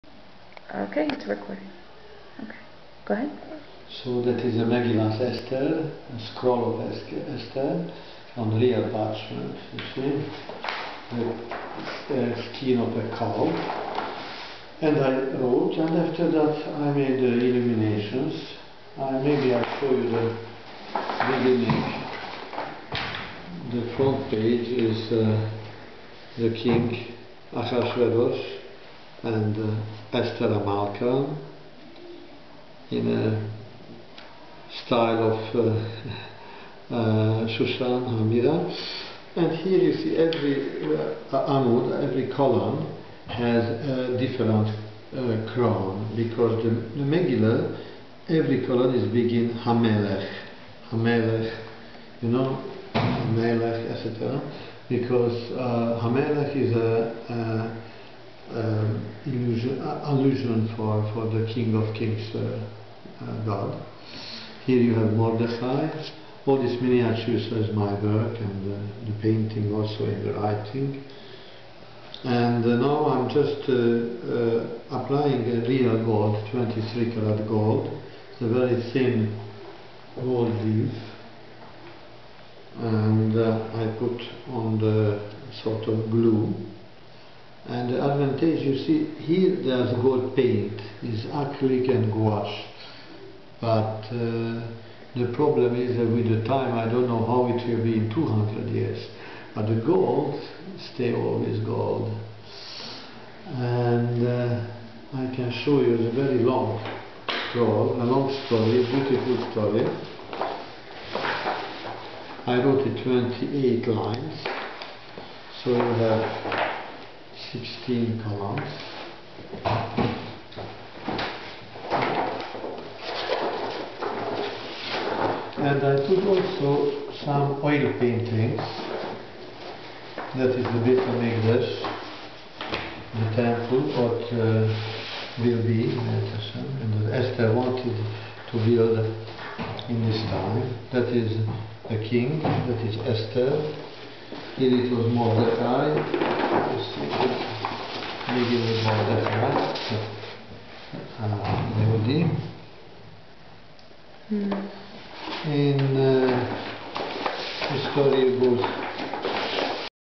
Rabbi Eliezer Reiner - Sofer STaM - Art Illuminations
For the past two decades, Rabbi Reiner has perfected a special technique, using the ancient art of hand-crafted manuscript illumination. During the Middle Ages, the entire process was completed in a multi-step assembly line, using extensive team-work. One artist drew the design, while several others painted the manuscript, and the scribe wrote the lettering. Today, Rabbi Reiner uses the very same technique, completing every single phase of the process on his own.